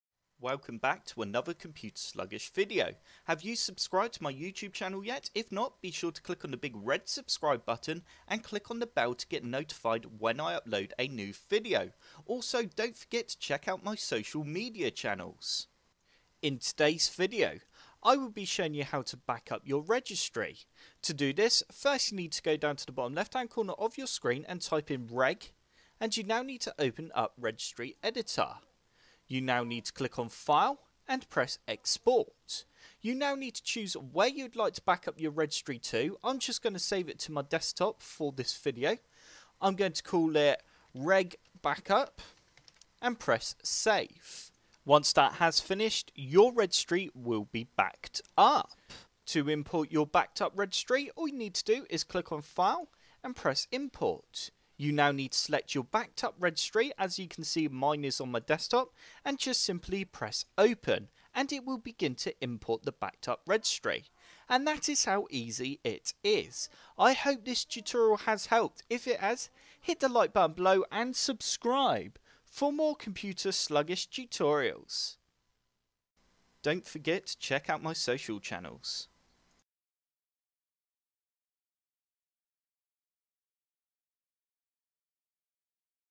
How To Backup and Restore Your Registry In Windows 10 Tutorial
In this Windows 10 Tutorial I will be showing you how to Backup your Registry and then easily restore your Registry after backing it up just in case it corrupts or something happens and you lose all your data.

Your Registry is important as it is an archive to collect and store the configuration settings of installed Applications, Hardware & Software, Components and many other things on your Windows 10 system.